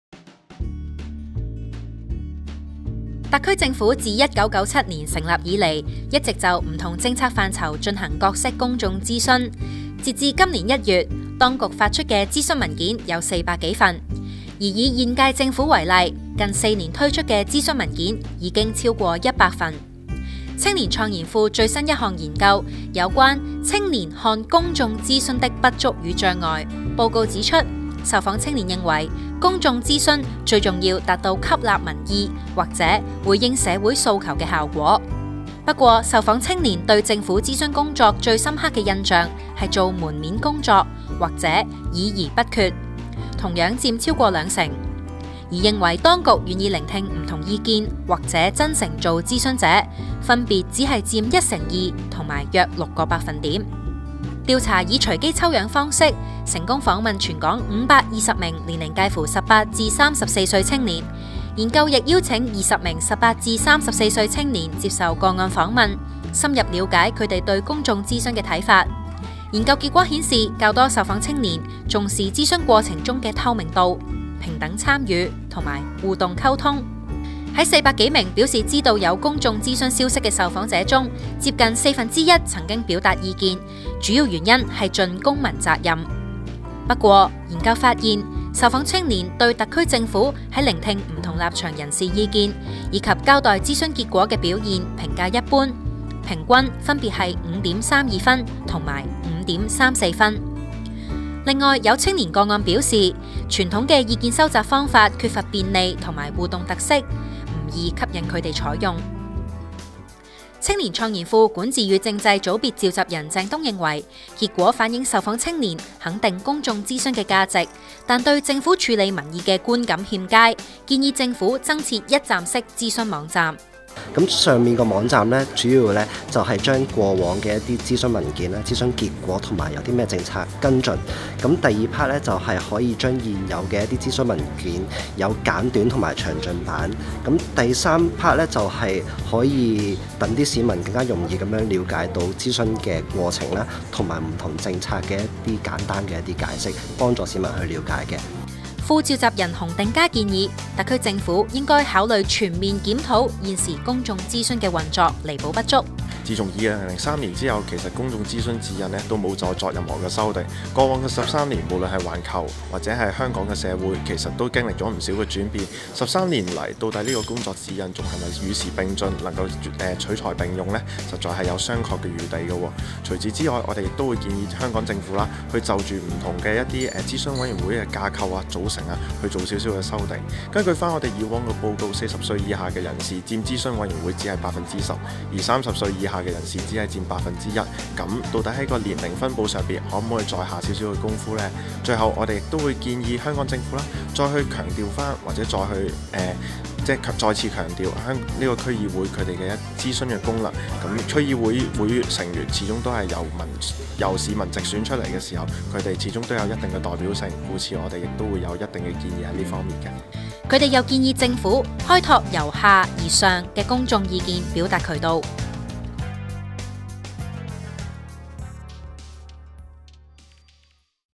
「青年創研庫」管治與政制組別 - 青年看公眾諮詢的不足與障礙
特區政府自1997年成立以來，一直就不同政策範疇進行各式公眾諮詢。截至今年1月，當局發出的諮詢文件有400多份。而以現屆政府為例，近4年推出的諮詢文件已超過100份。「青年創研庫」最新一項研究有關「青年看公眾諮詢的不足與障礙」，報告指出，受訪青年認為公眾諮詢最重要達至吸納民意或回應社會訴求的效果。

製作：M21